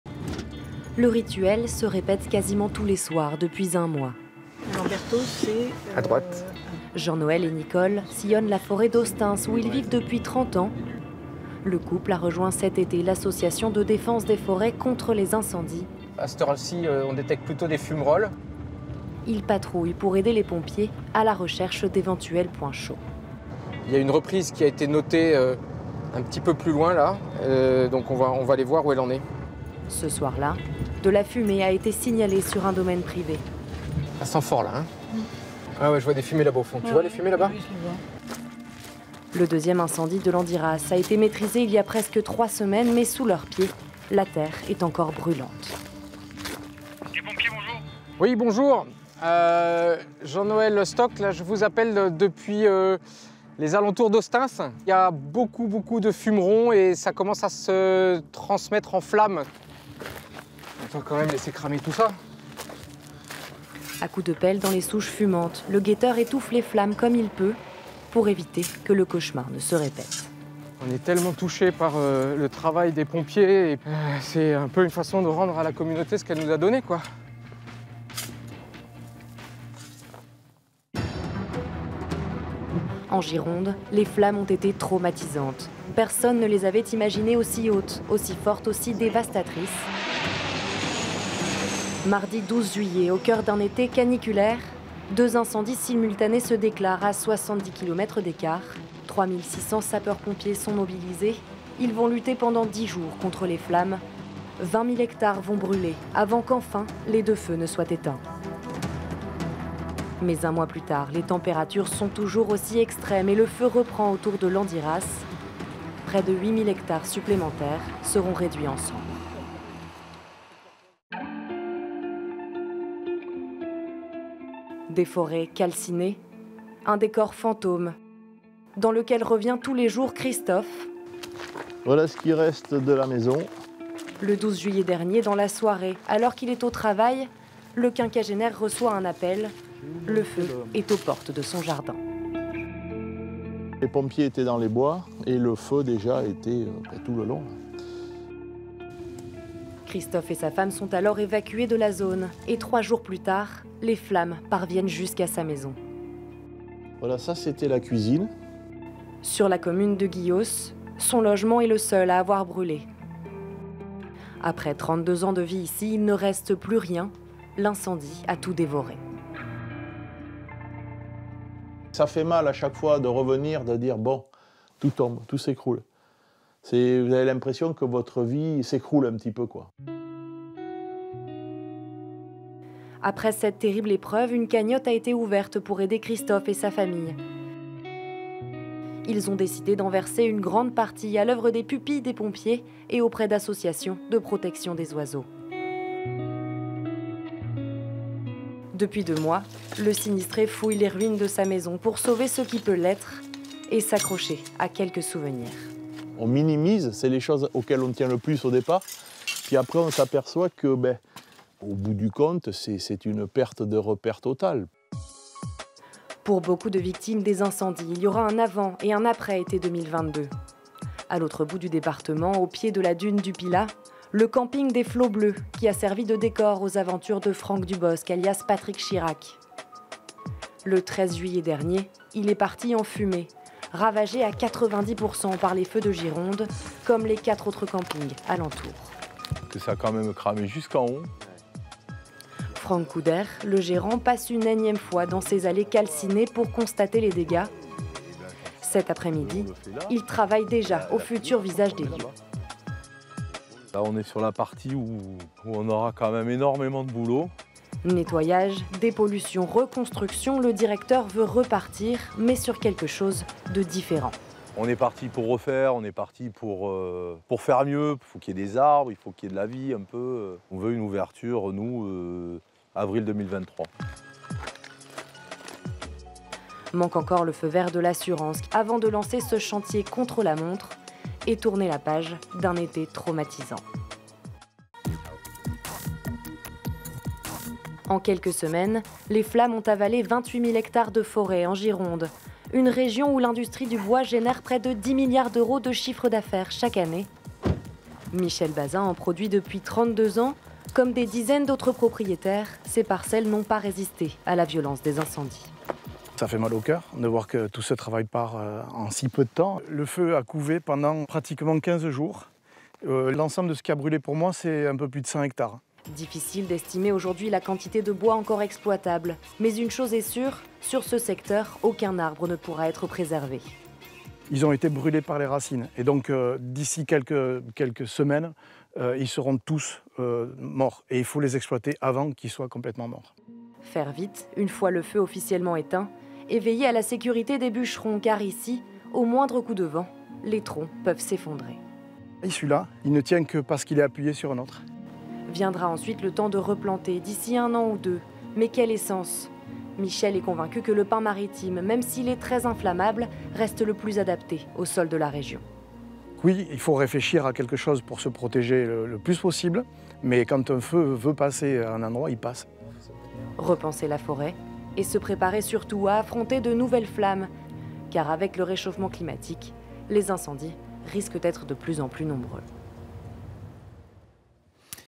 MAG - Incendies en Gironde : renaître de ses cendres
Retour en Gironde après les violents incendies de l'été 2022. Ecriture et montage de ce reportage diffusé en septembre 2022 dans le 12/45 d'M6.